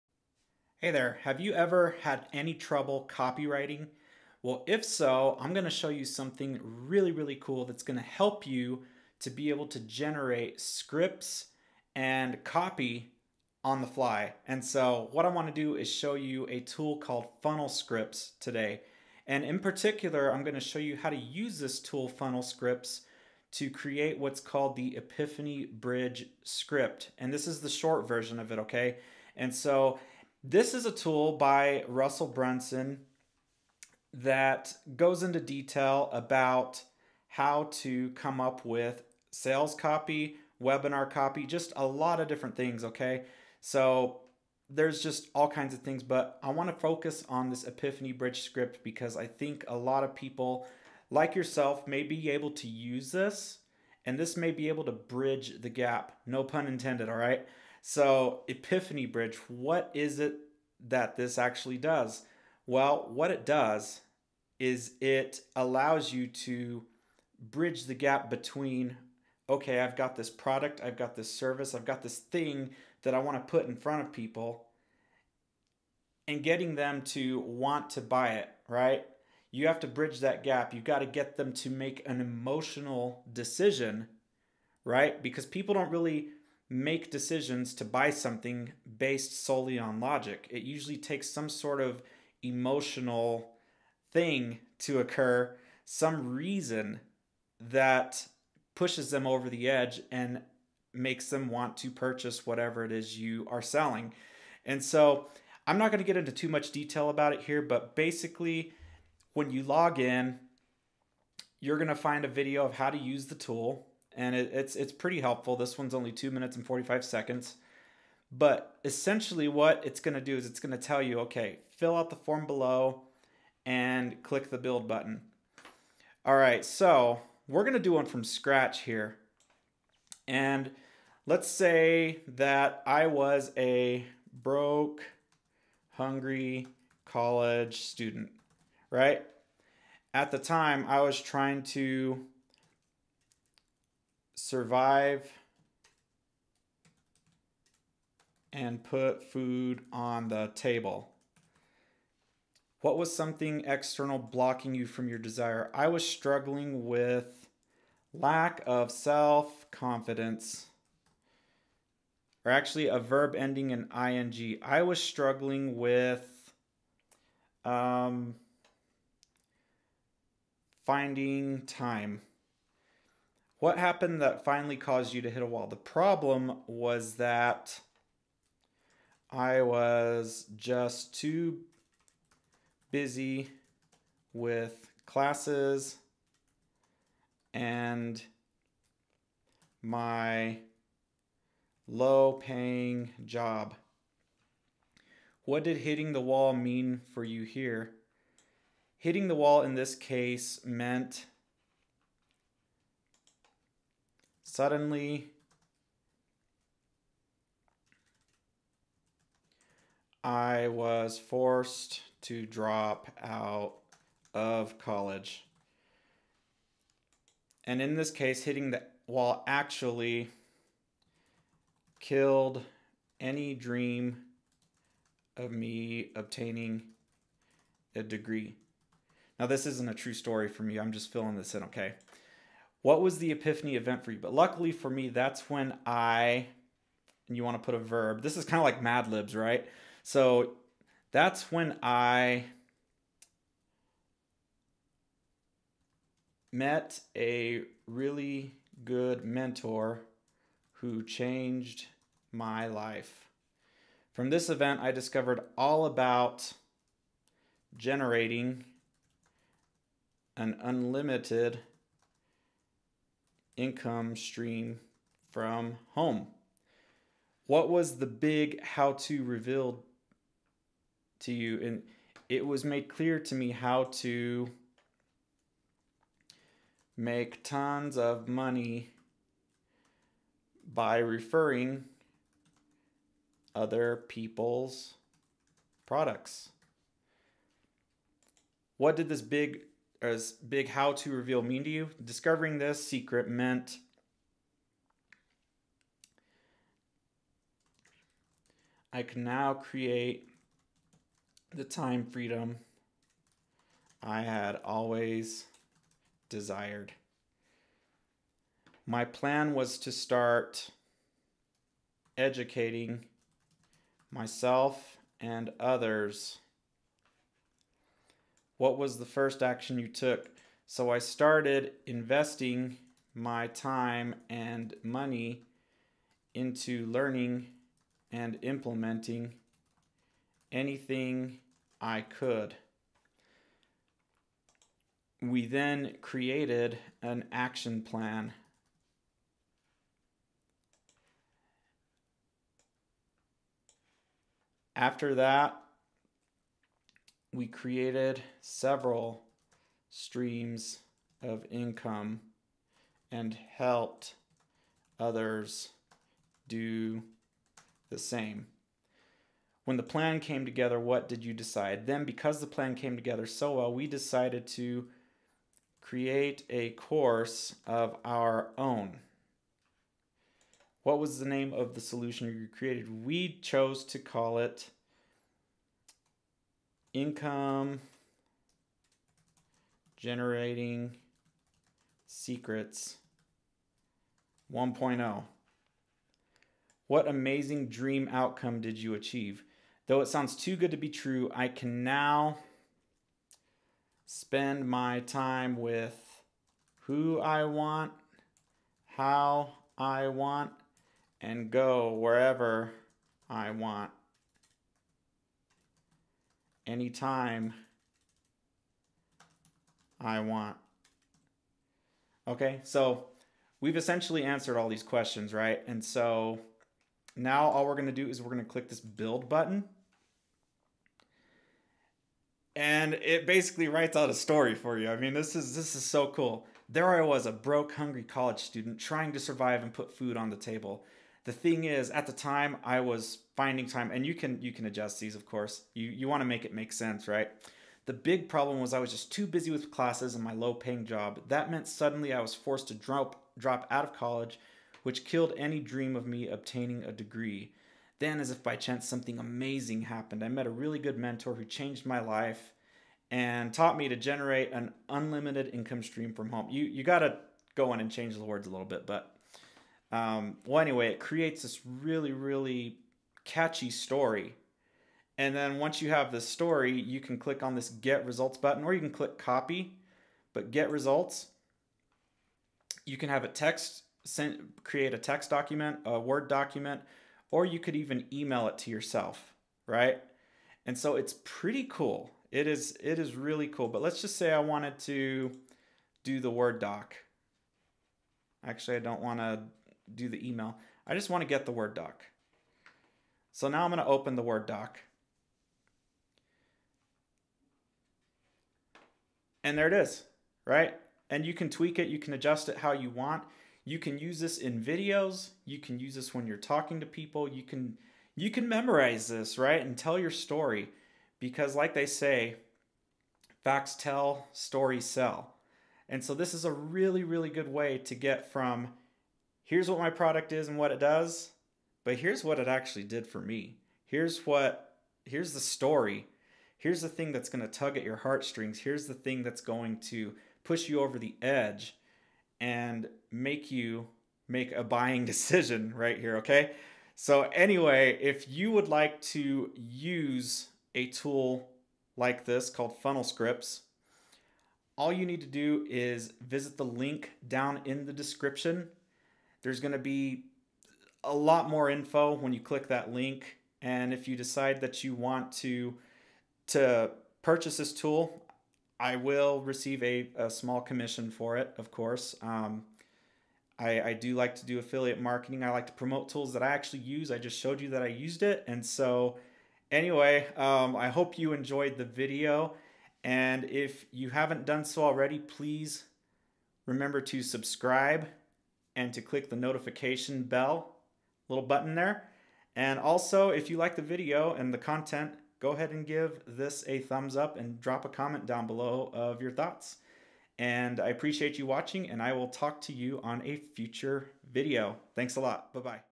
Funnel Scripts | Epiphany Bridge Script (Short) Demo
RESOURCE LINKS

In this video I'm going to show you how to generate an Epiphany Bridge Script using the Funnel Scripts tool.  This is covered in Russell Brunson's book "Expert Secrets" and is very useful for getting high sales conversions.